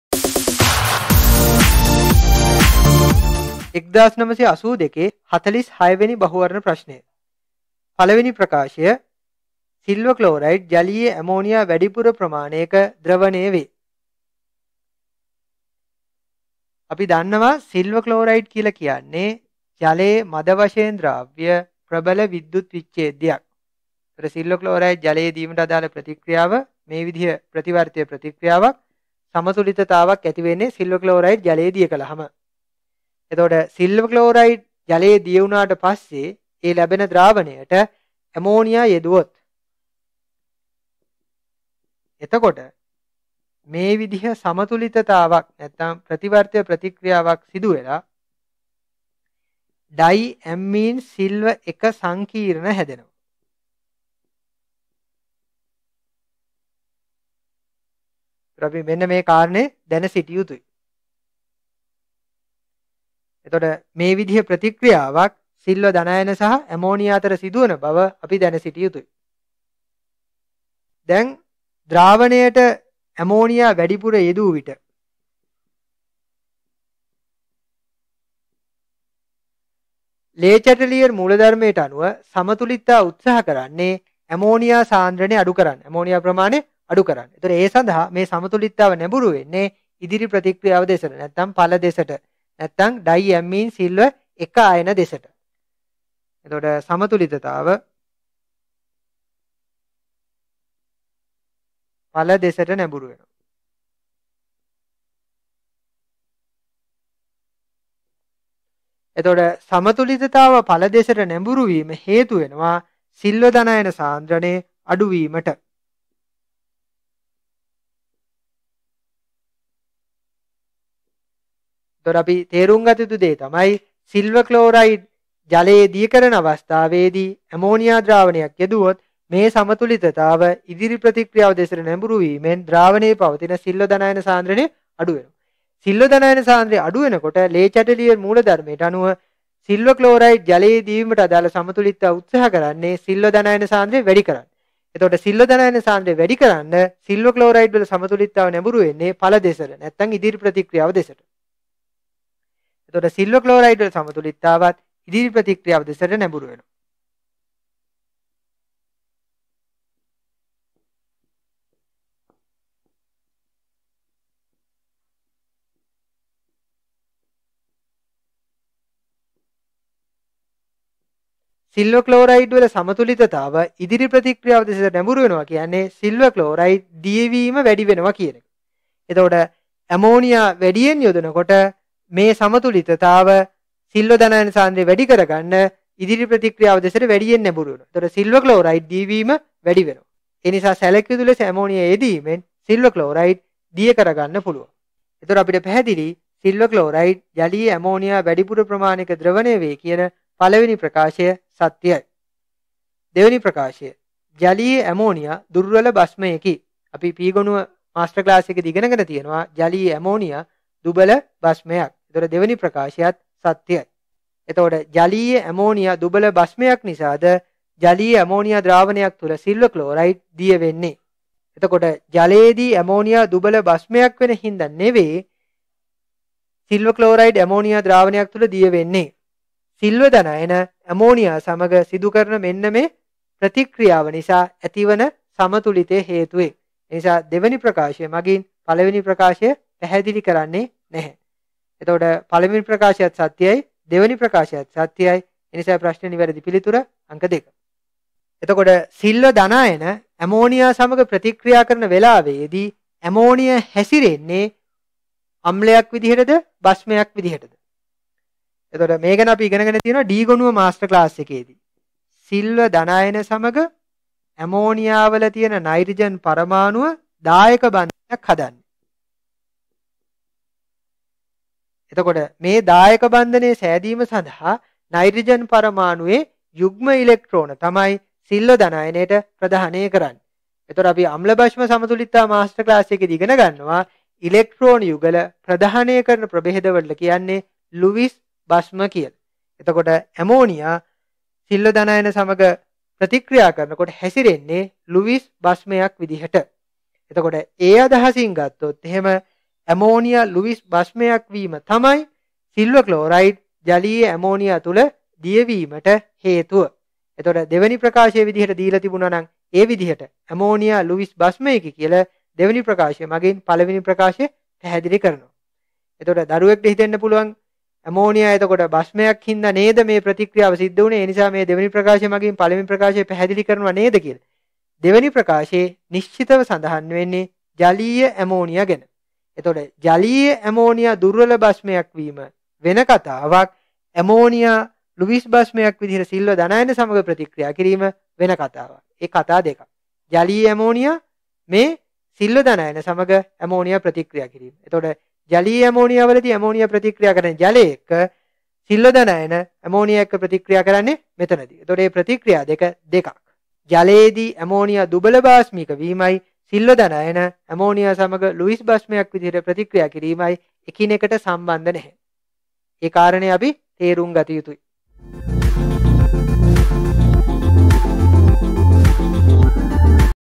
Chemistry Past Papers: A/L 1982 MCQ 46 (Sinhala)
1980 සිට මේ දක්වා ද්‍රාව්‍යතා ගුණිතය ප්‍රශ්න මෙතැනින් download කර ගන්න.

අධ්‍යයන පොදු සහතික පත්‍ර (උසස් පෙළ - A/L) රසායන විද්‍යාව සිද්ධාන්ත ඉගෙනීමට

උසස් පෙළ රසායන විද්‍යාව 1982 46 වන බහුවරණ ප්‍රශ්නය සඳහා අමිල සේනාධීර ඉදිරිපත් කළ විවරණය

This cover has been designed using resources from Freepik.com

රසායන විද්‍යාව බහුවරණ විවරණය
රසායන විද්‍යාව සම්පත් පොත
රසායන විද්‍යාව ප්‍රායෝගික පරීක්ෂණ
රසායනික ගණනය කිරීම් pdf